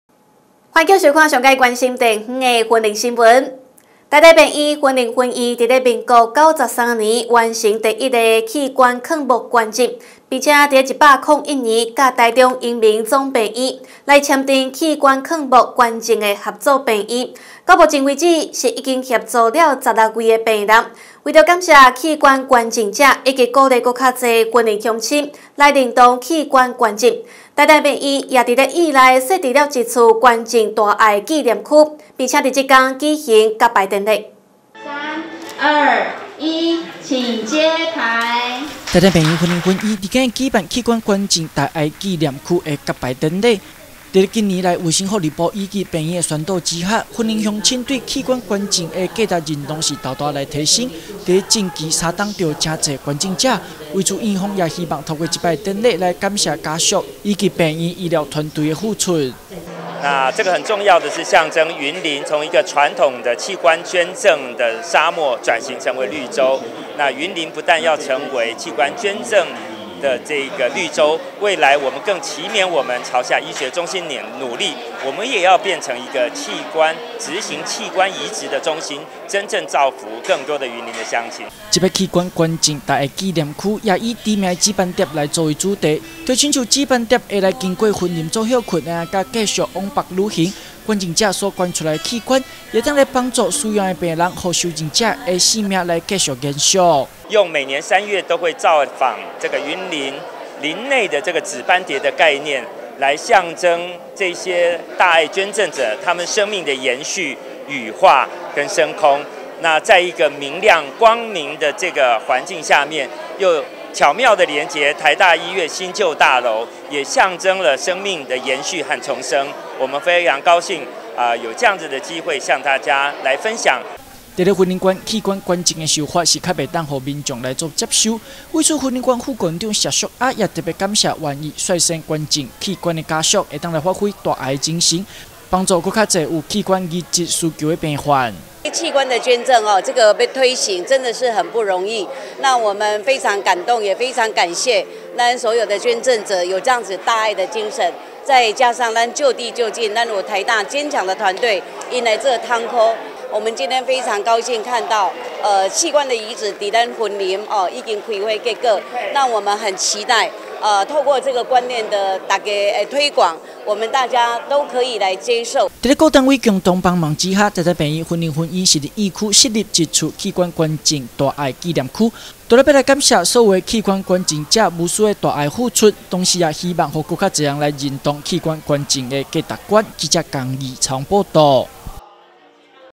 緬懷器捐者大愛 院方設立器捐大愛紀念區-雲林新聞網
臺大醫院雲林分院於民國93年，完成第一例的器官勸募捐贈，並在101年與台中榮民總醫院，簽訂為器官勸募捐贈合作醫院，至今為止是已經協助了16位病人成就大愛，而為了感謝器官捐贈者，以及鼓勵更多雲林鄉親認同器官捐贈的價值觀，臺大醫院也在院內，設立了一處捐贈大愛紀念區，並在今日舉行揭牌典禮。